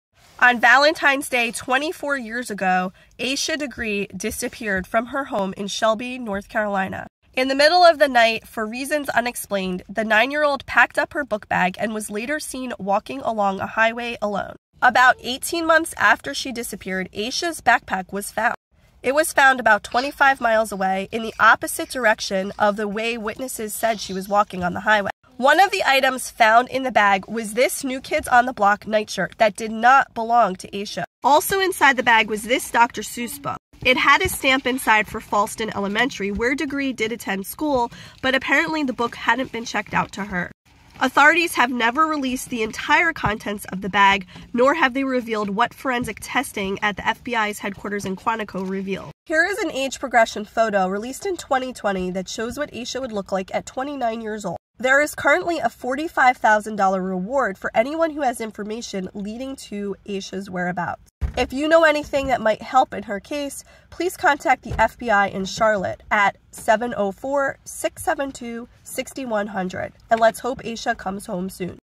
Vanished on Valentine's Day 💔 The Disappearance of Asha Degree 😢
Vanished on Valentine's Day 💔😢

24 years ago today, 9-year-old Asha Degree vanished from Shelby, North Carolina.

18 months later, Degree's backpack was found about 25 miles from where she was last seen walking along the highway alone in the middle of the night. The bag contained items that did not belong to her.

There is a $45,000 reward for information leading to Asha Degree's whereabouts.